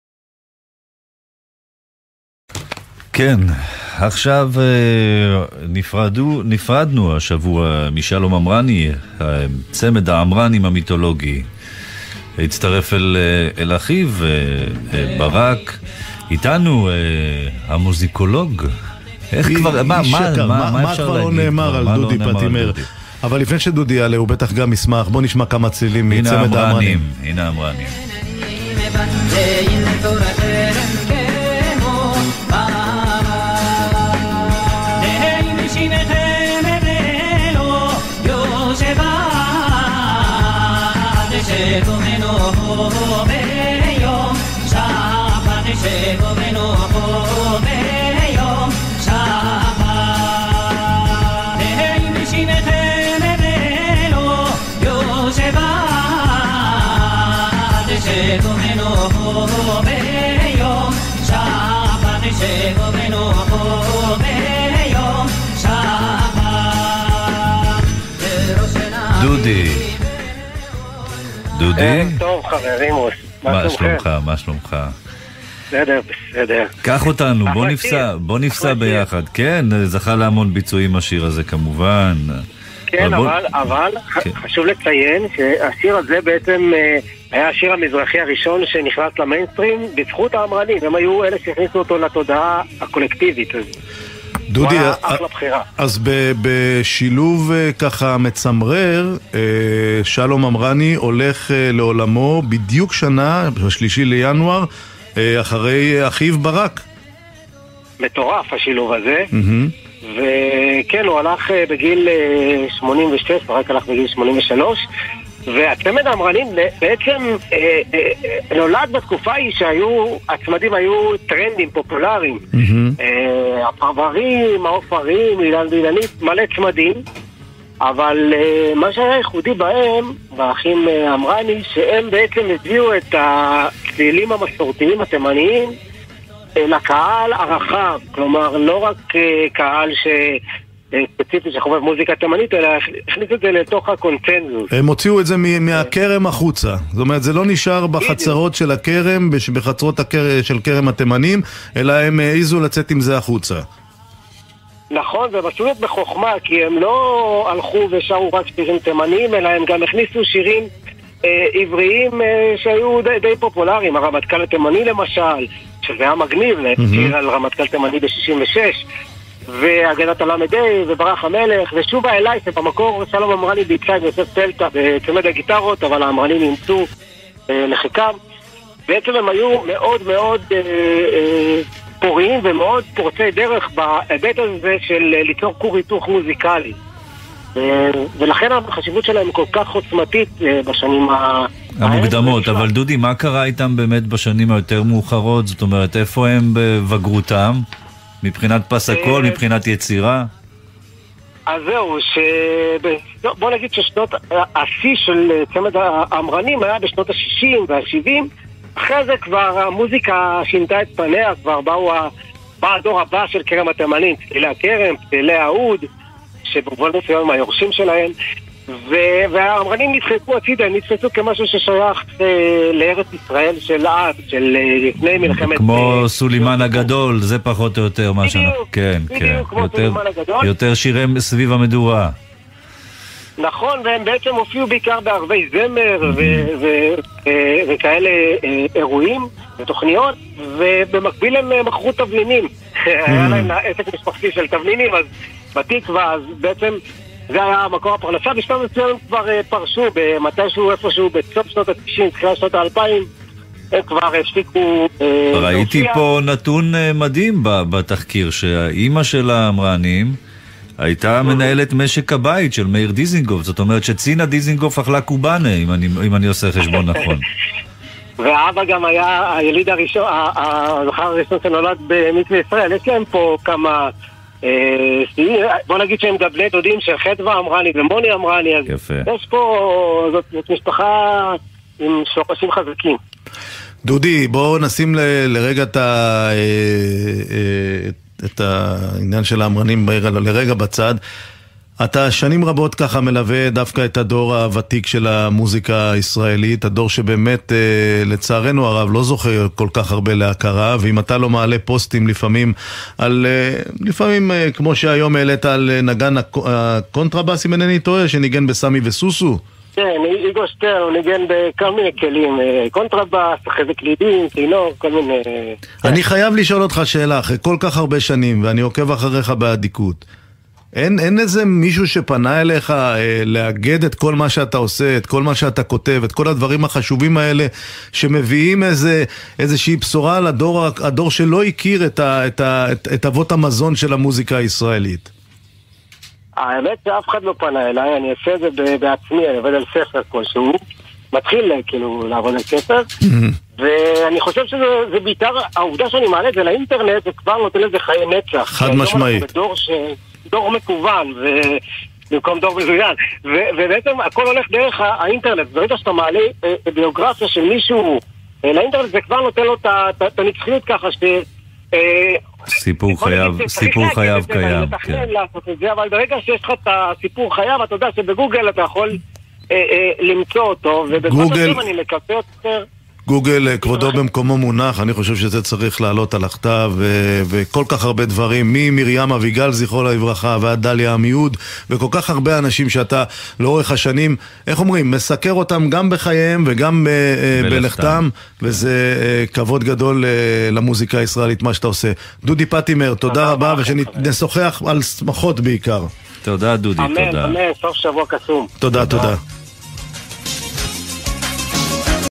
דודי פטימר בראיון לבן וקובי פרג' בגלי צה"ל - צמד העמרנים ותיעוד מוזיקה
הראיון שודר בתאריך ה-5 בינואר 2023.